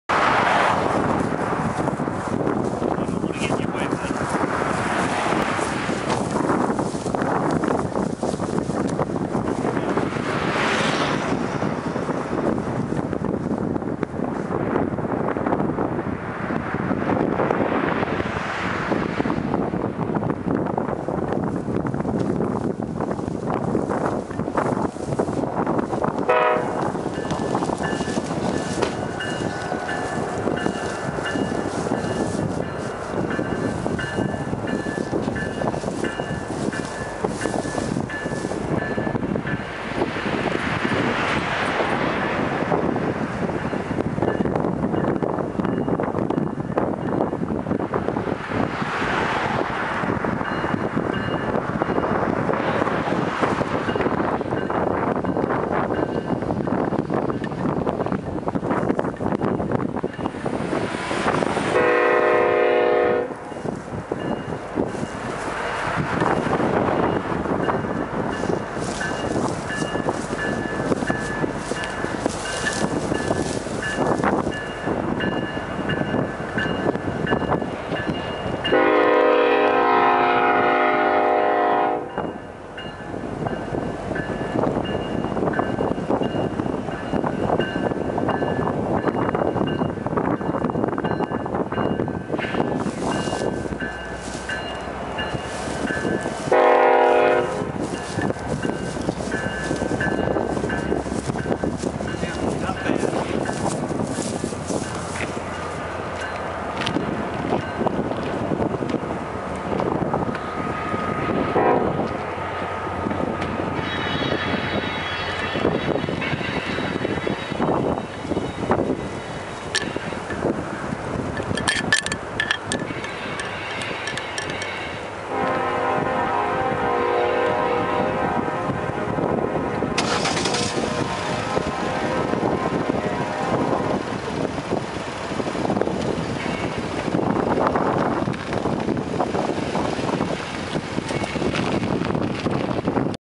HD: 3/12/13 NS 1071 Lady Liberty Going Across Horseshoe Lake
Skip to 00.25. After getting shots of NS 1071 in Mitchell, IL a group of us railfanners went out to Horseshoe Lake in Fairmont City, IL to get some more shots of it going across the lake. Soon as I got enough shots of this train I went home.

BNSF: U-MNUMIJ4-09
NS: SD70ACe 1071 Central of New Jersey
NS: SD70ACe 1000
NS: SD70ACe 1025